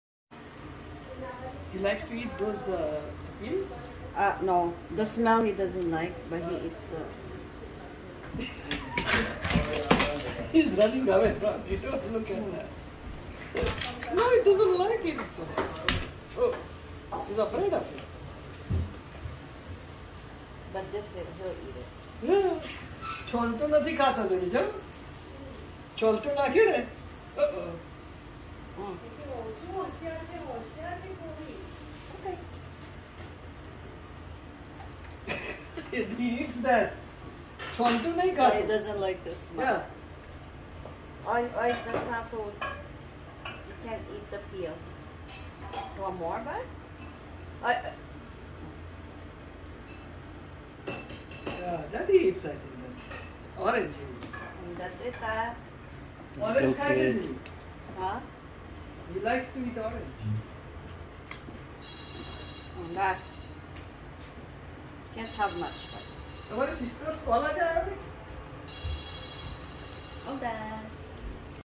Kovy eating 🍊
Kovy likes to eat  oranges but doesn't like the smell of it.